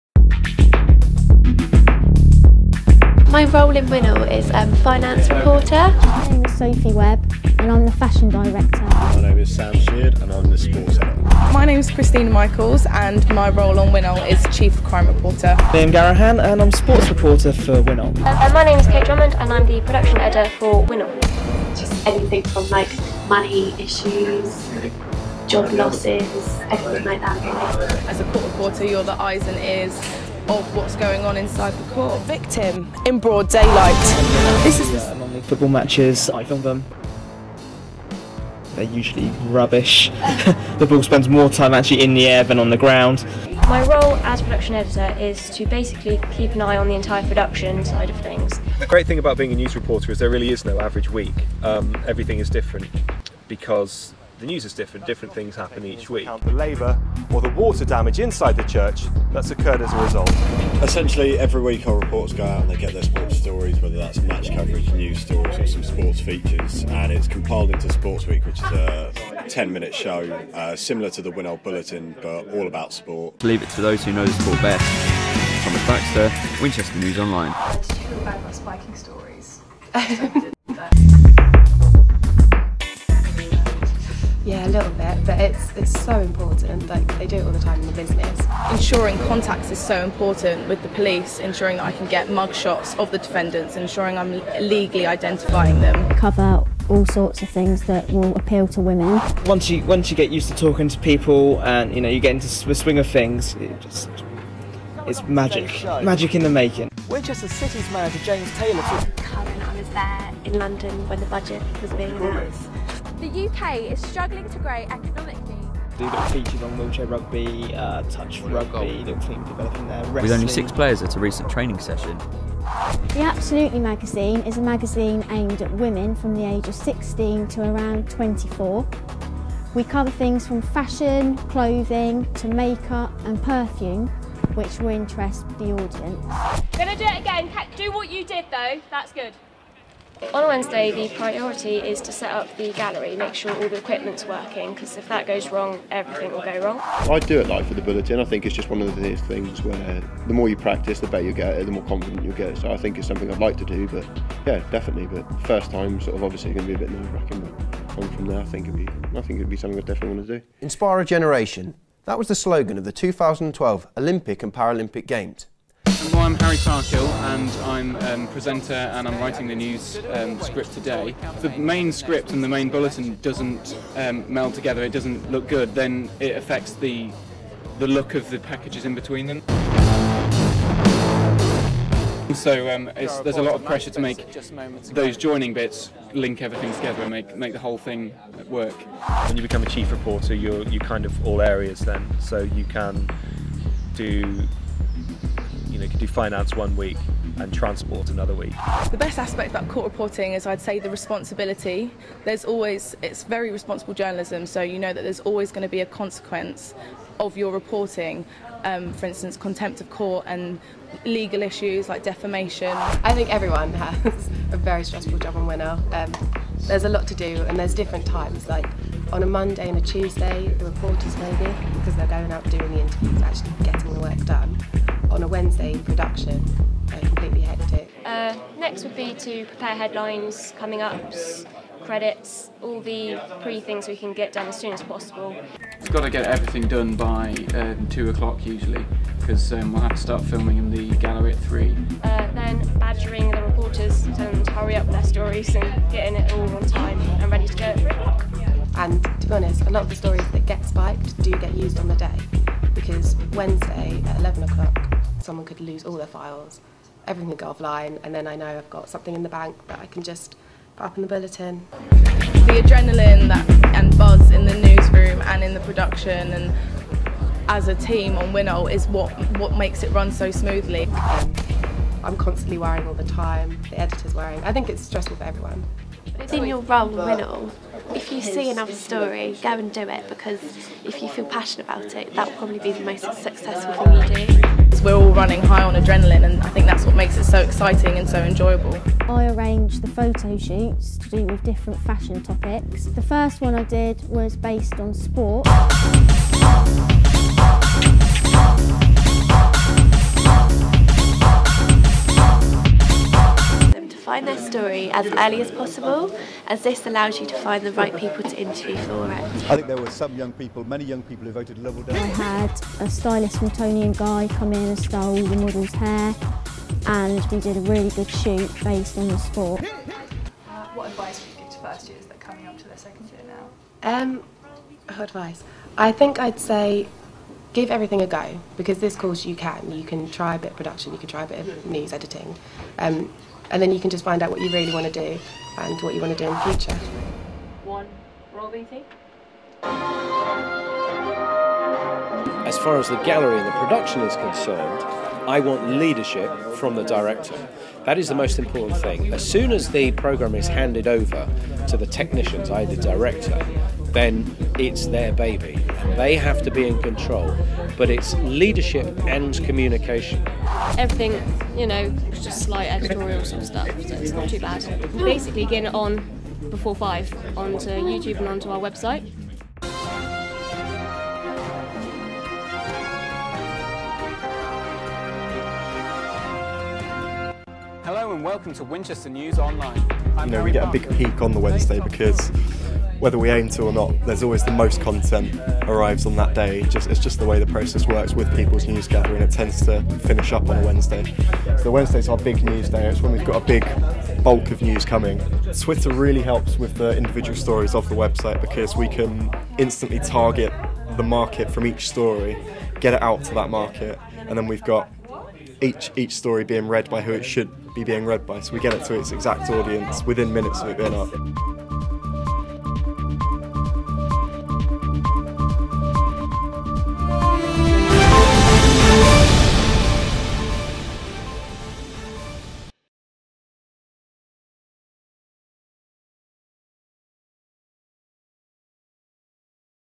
MA JOURNALISM
Students on the journalism courses at the University of Winchester talk about their roles on Winchester News Online (WINOL) the live workplace simulation project ewhich forms the core of the MA journalism and BA journalism courses.

WINOL is recognised by the BBC for its innovation in journalism education, for the excellence of he work created by students on the journalism courses.

By first year students and lecturers on the journalism courses at the University of Winchester near London in the UK.